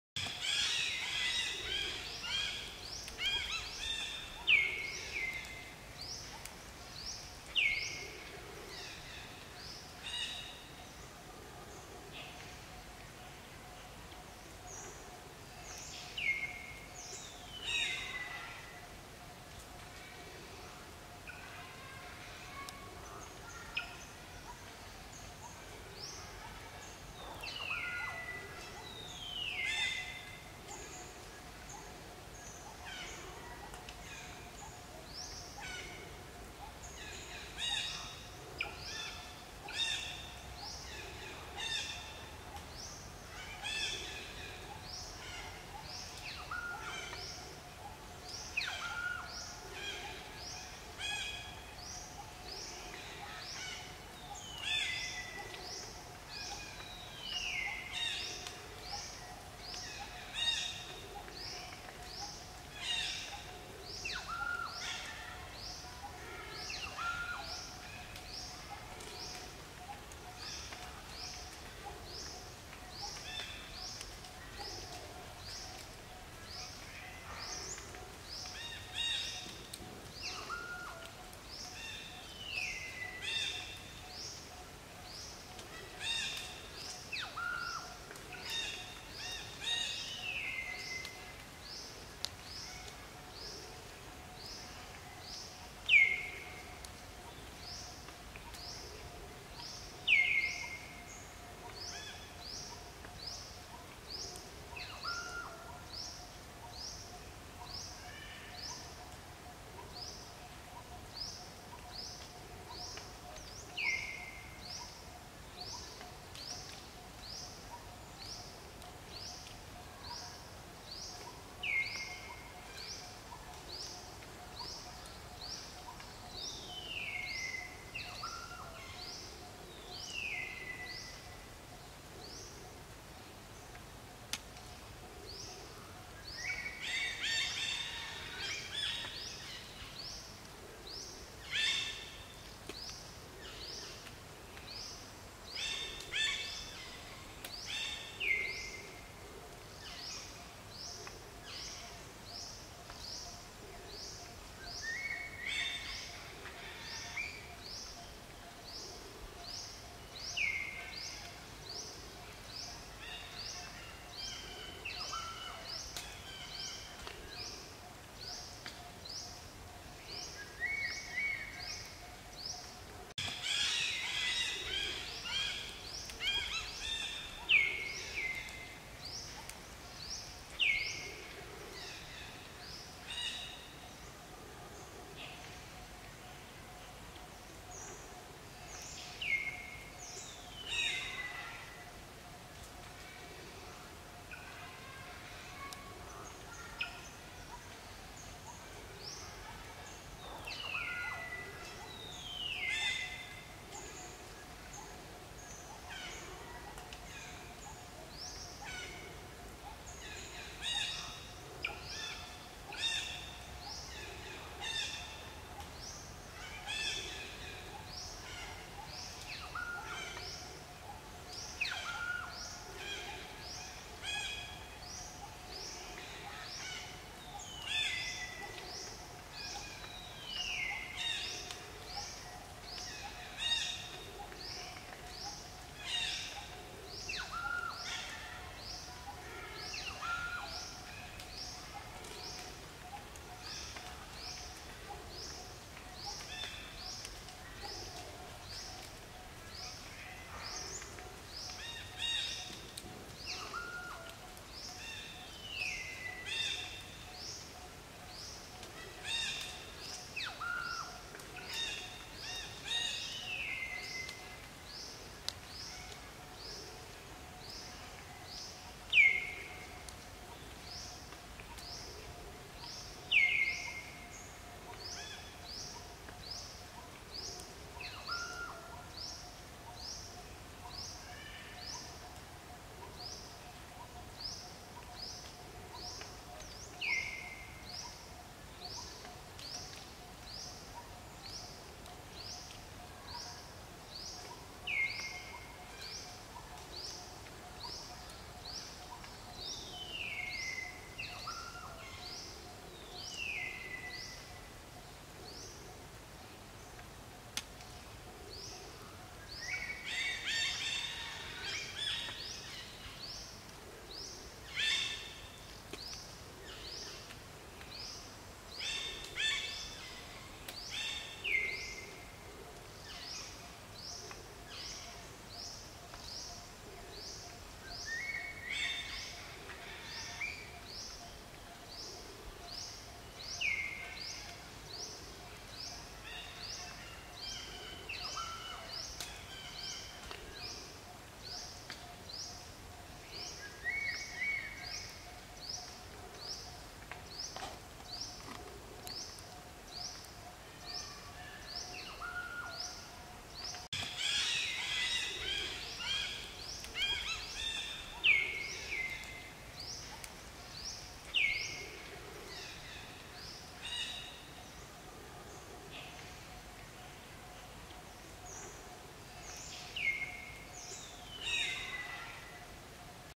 Mom Brianna Pour Milk On The Head Of Brindy During Mom Drinking It and She Eats A Lots Mangos
Welcome to channel: @realmonkey2023
Mom Brianna Pour Milk On The Head Of Brindy During Mom Drinking It
Other Reaction!
You can watch it even when you aren't connected.